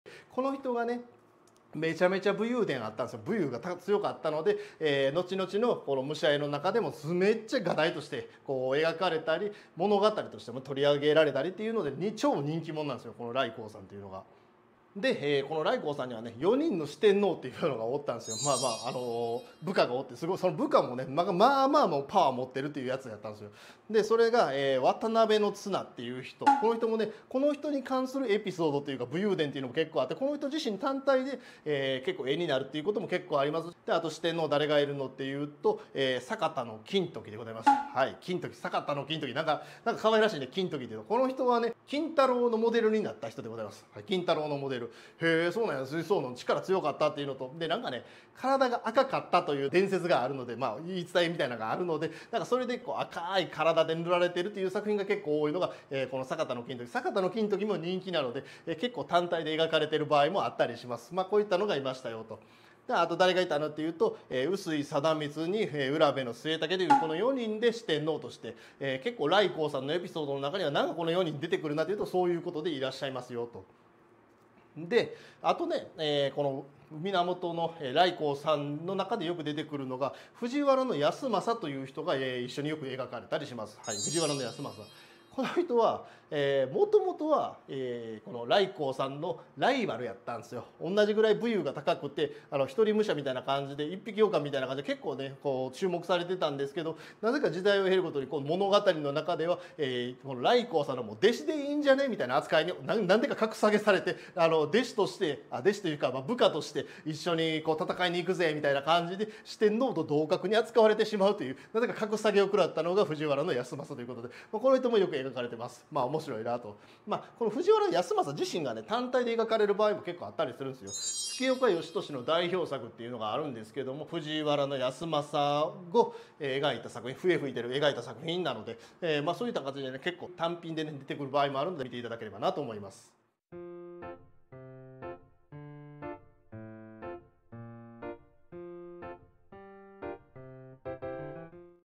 武者絵で良く描かれる源頼光と四天王（渡辺綱、坂田金時、碓井貞光、卜部季武）と藤原保昌 | 日本美術、アート
武者絵で良く描かれる源頼光と四天王（渡辺綱、坂田金時、碓井貞光、卜部季武）と藤原保昌について解説しています。

■浮世絵 武者絵 アート解説 | ボストン美術館所蔵「THE HEROES　刀剣ｘ浮世絵－武者たちの物語」展覧会

株式会社 野村美術
〒655-0021
兵庫県神戸市垂水区馬場通7-23